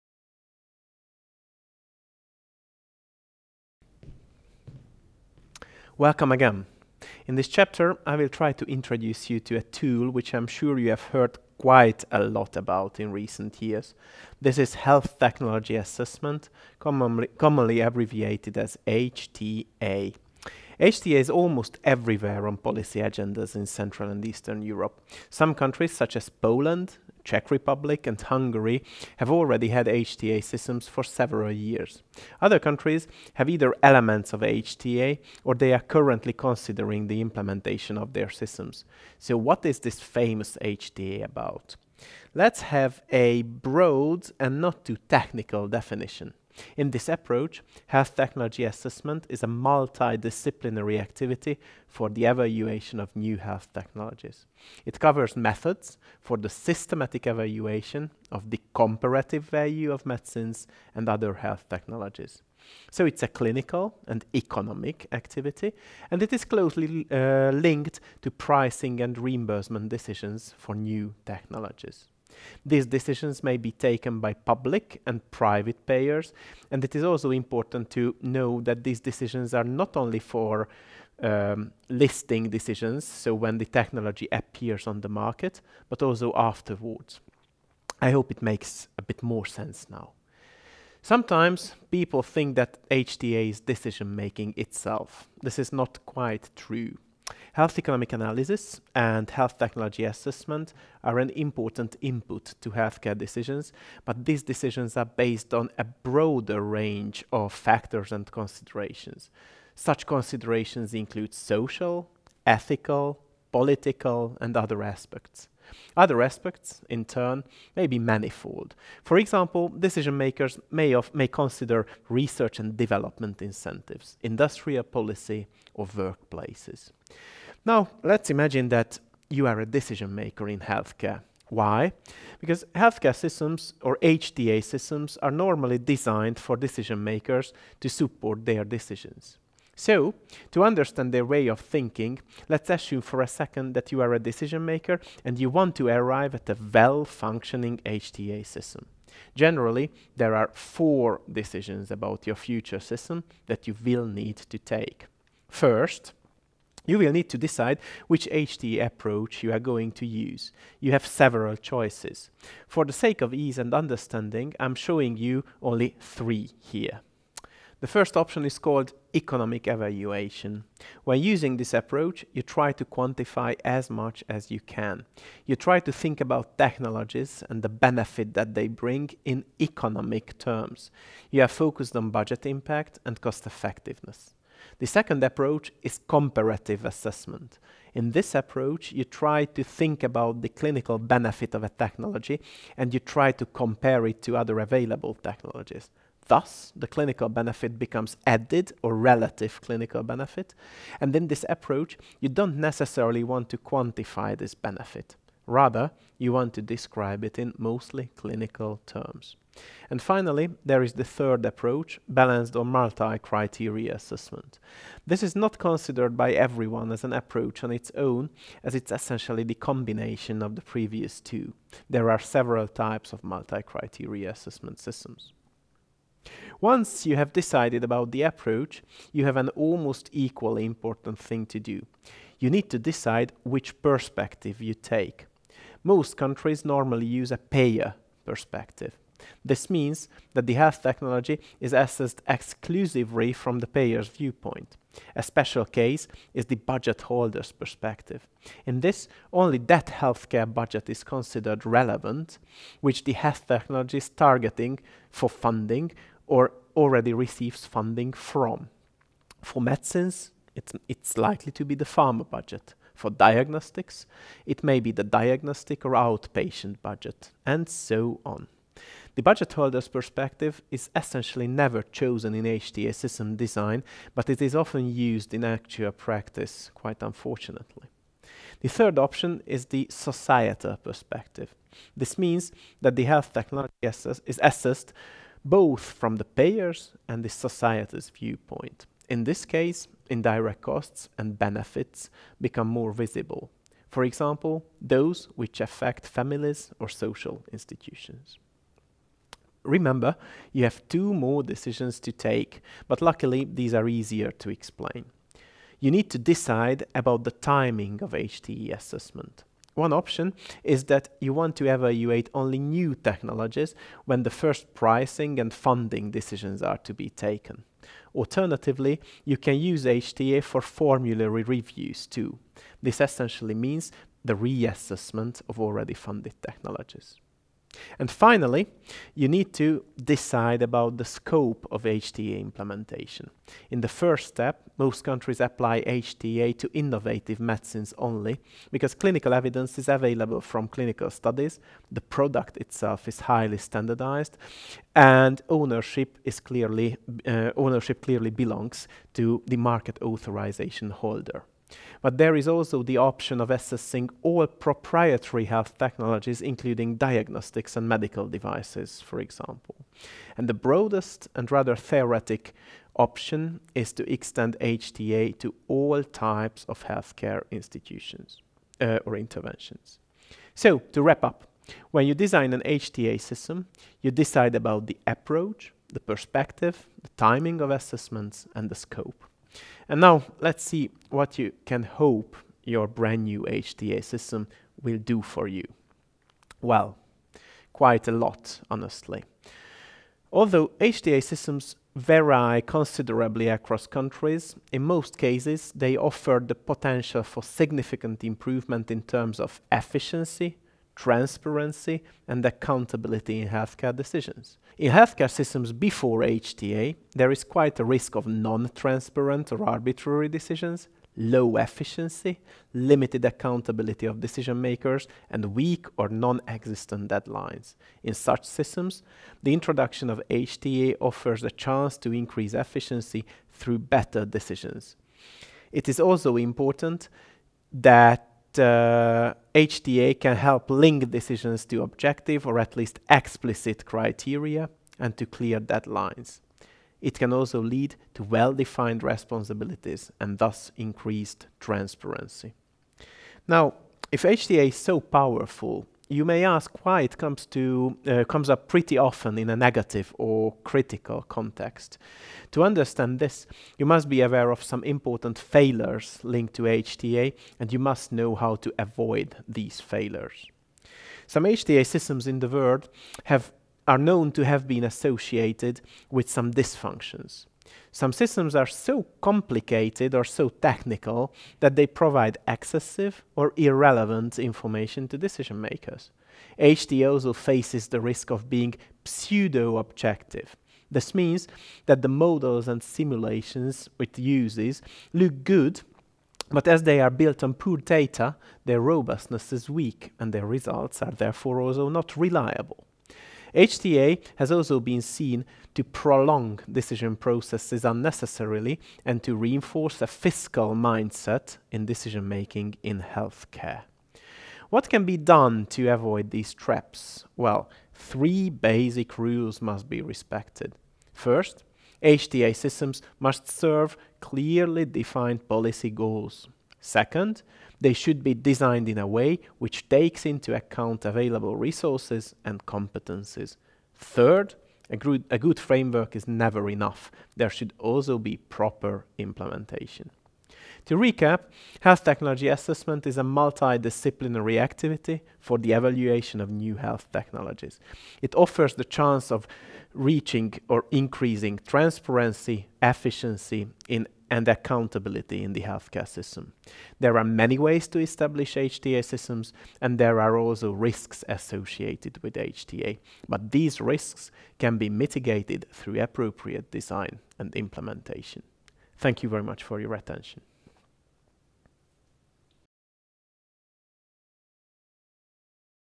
Goals and Purpose of Health Technology Assessments ( HTA)
Do you want to know more on the evaluation of medicines and medical devices? Have a look at this video and learn more about  Health Technology Assessments ( HTA)benefits and risks.